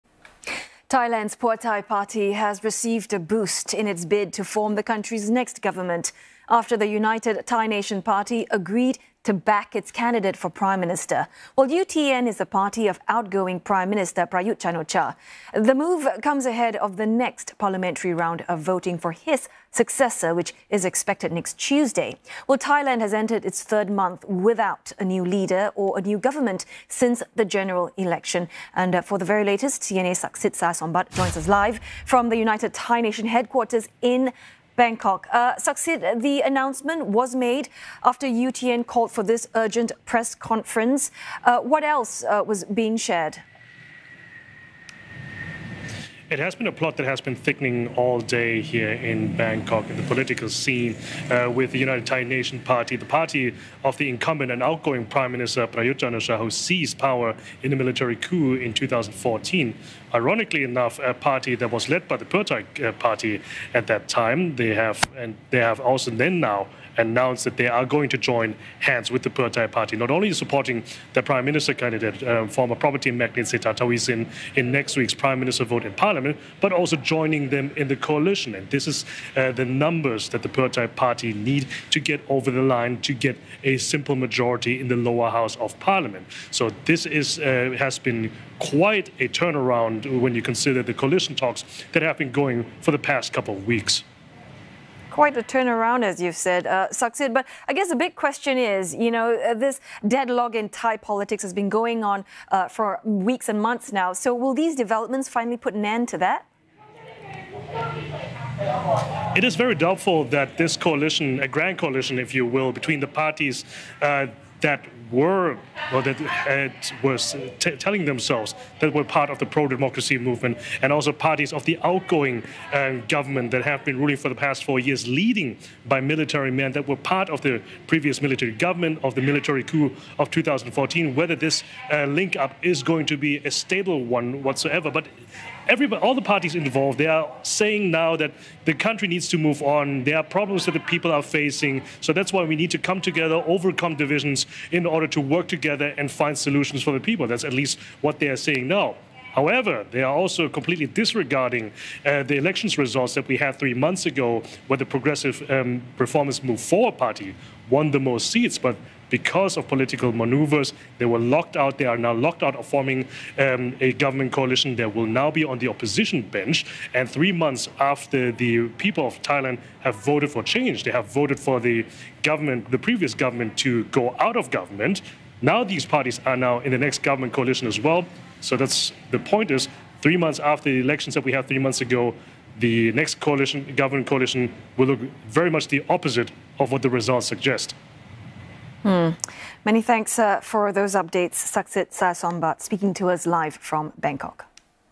United Thai Nation to back Pheu Thai in PM vote on Aug 22: Spokesperson
Thailand's Pheu Thai Party has received a boost in its bid to form the country's next government, after the United Thai Nation Party (UTN) agreed to back its candidate for prime minister. UTN is the party of outgoing Prime Minister Prayut Chan-o-cha. The move comes ahead of the next parliamentary round of voting for his successor, which is expected to take place on Aug 22. Thailand has entered its third month without a new leader or a new government since the general election.